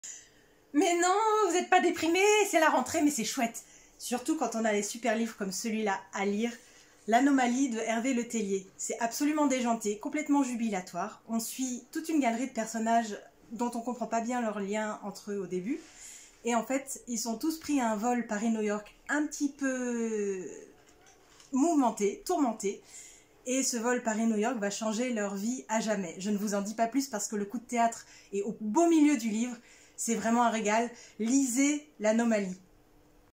[Rentrée Littéraire 2020] L'Anomalie, Hervé Le Tellier
Nouveauté 2020, vos libraires se sont amusées à vous présenter en 30 secondes leurs coups de cœur de la rentrée !
Top chrono, c'est parti, aujourd'hui Magali vous parle de L'Anomalie d'Hervé Le Tellier (Gallimard, 2020)